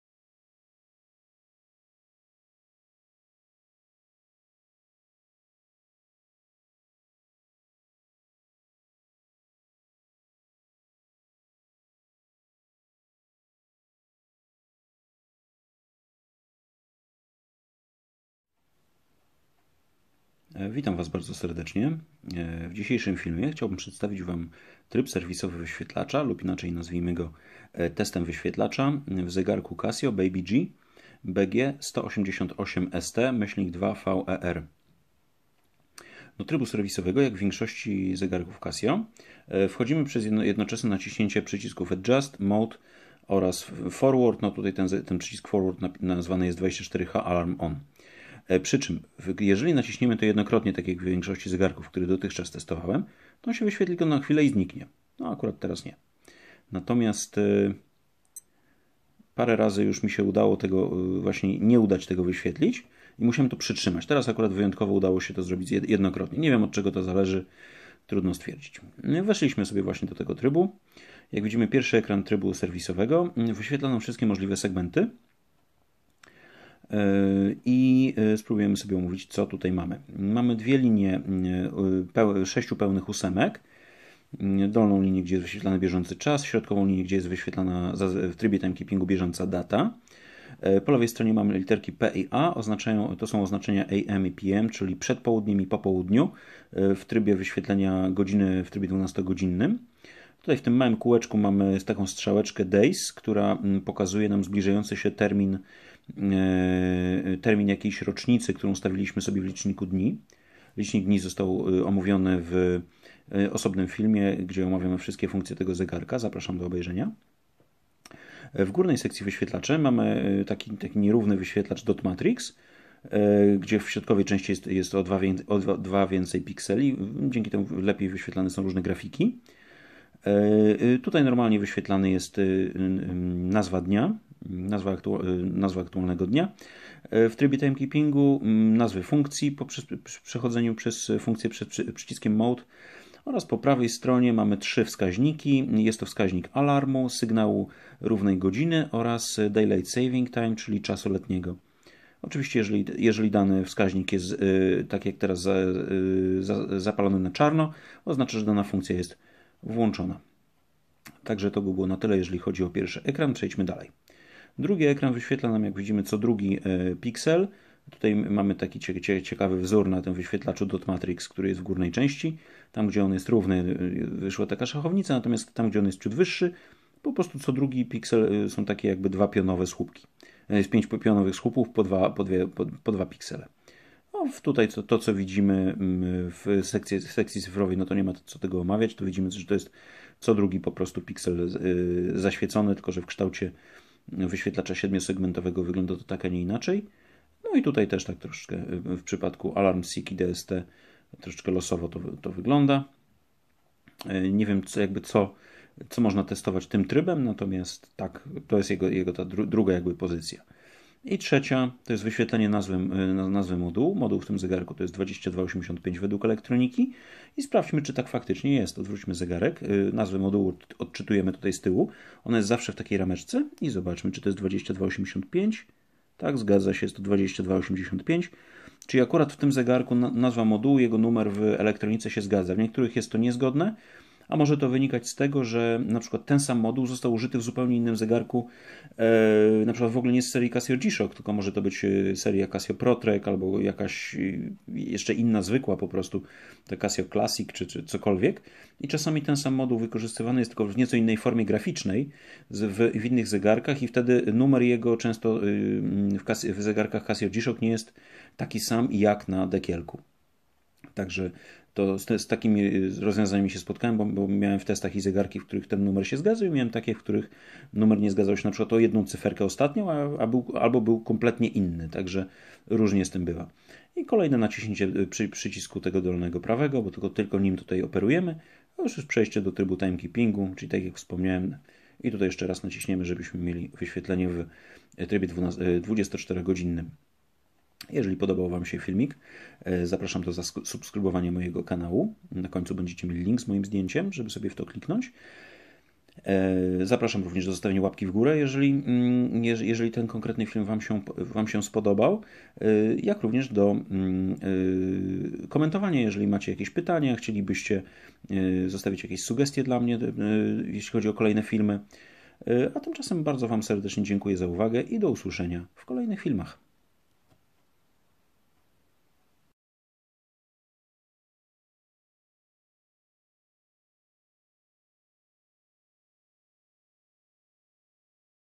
Casio Baby-G BG-188ST-2VER - tryb serwisowy [PL]
Zapraszam do obejrzenia mojej prezentacji zegarka Casio Baby-G BG-188ST-2VER, który testuję dzięki uprzejmości kolegi Tomka.
Pokazuję w niej, jak wejść do trybu serwisowego. Moja wiedza na temat możliwości wykorzystania tego trybu jest niekompletna, także jeśli wiecie coś więcej na ten temat, zapraszam do podzielenia się w komentarzach.

Jeśli macie jakieś pytania odnośnie tego modelu, chętnie odpowiem.